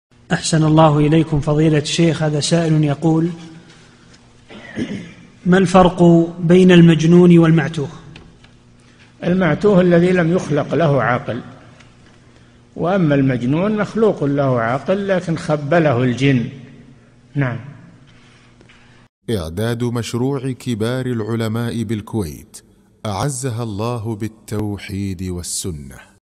[2900 -3022] ما الفرق بين المجنون والمعتوه؟ - الشيخ صالح الفوزان
-"[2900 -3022] السؤال: ما الفرق بين المجنون والمعتوه؟
الجَوَابُ: المعتوه الذي لم يخلق له عقل، وأما المجنون مخلوقٌ له عقل لكن خبله الجن.
للاشتراك في قناة الشيخ صالح الفوزان

للاستفسار والإقتراحات والدعم
رئيس الفريق
لمشروع كبار العلماء